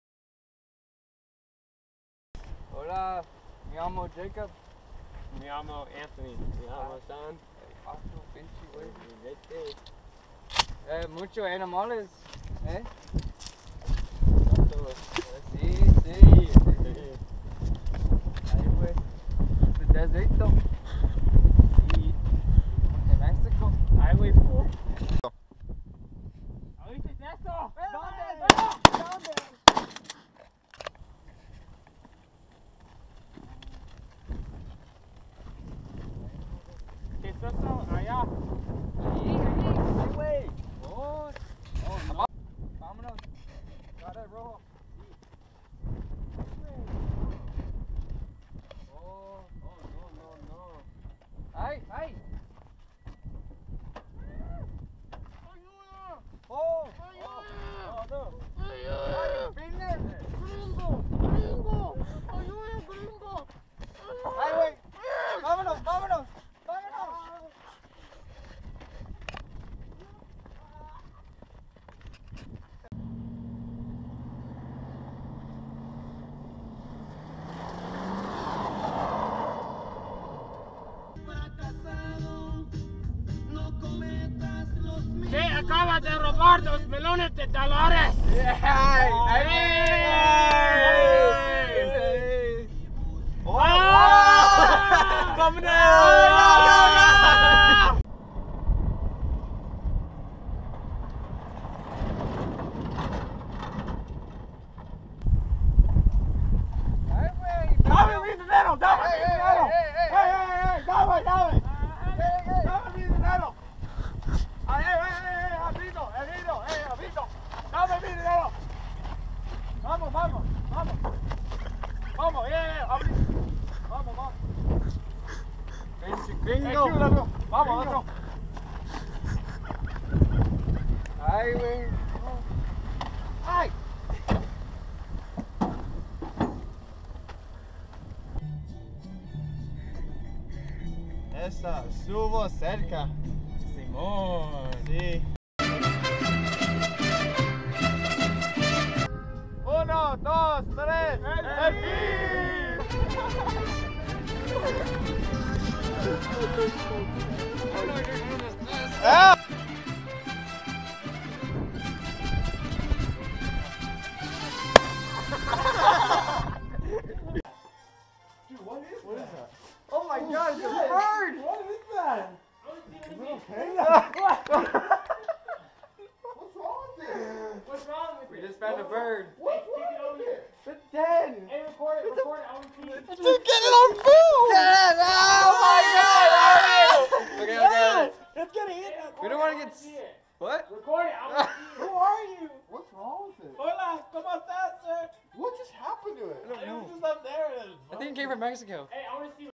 Spanish project
2 million dollar Briefcase
Starring Cameron, Jacob, Q, Anthony, Shawn, Erick, Gabriel, and Noe